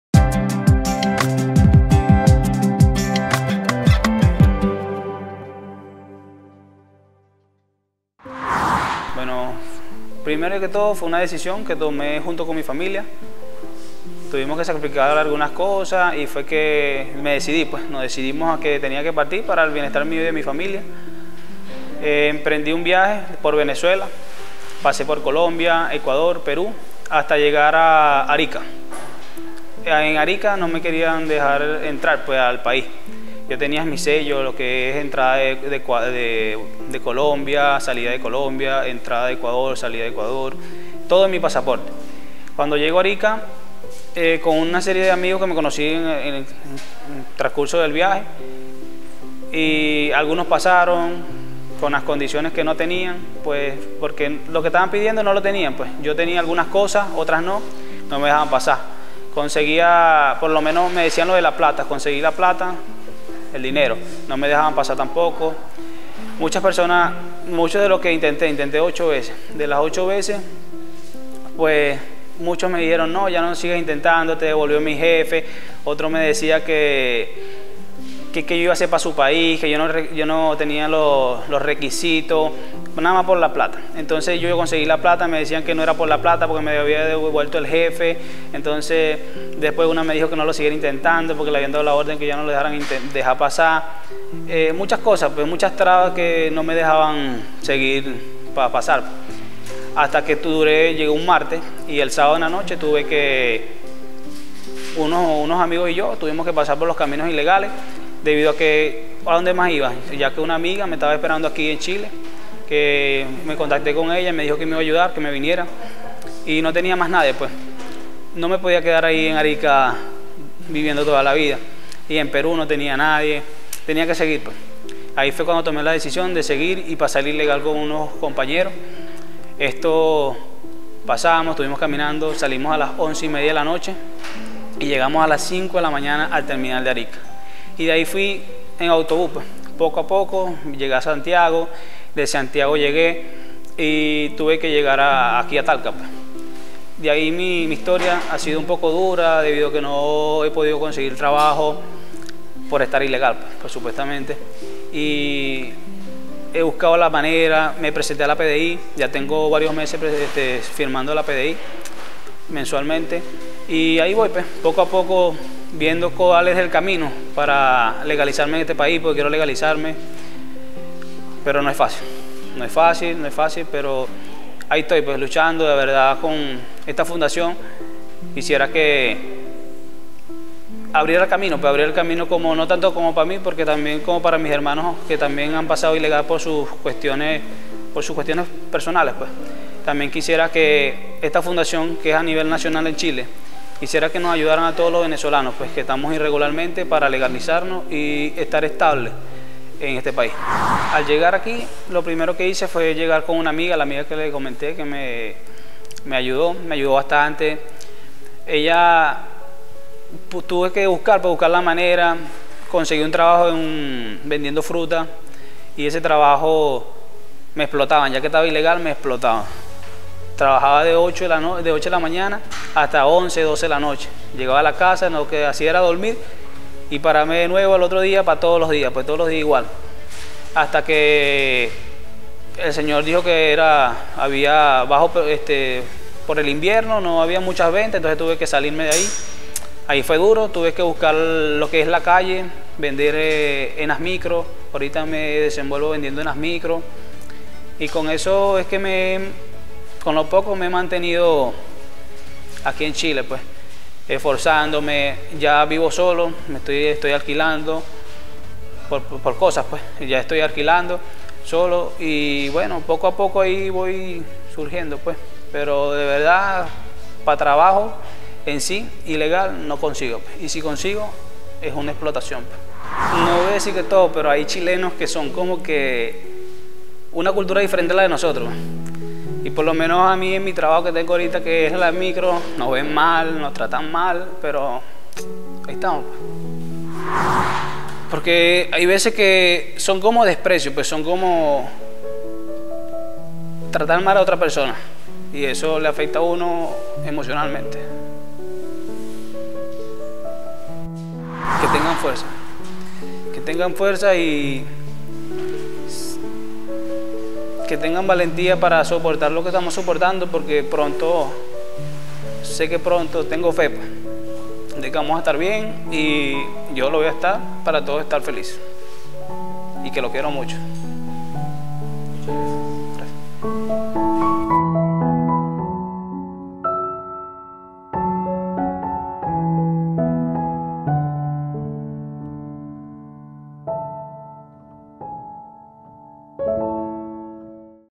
Historias de migrantes (Fernando-migrante venezolano)
Estas historias son un relato personal e intimo sobre la migración a nuestro país. El sentido que tiene es compartir un mundo de visiones y sentimientos de las personas que llegan. Este proceso es importante para los migrantes y para los chilenos, puesto que nos presenta una vivencia personal que bien pudiera ser la de cualquiera de nosotros.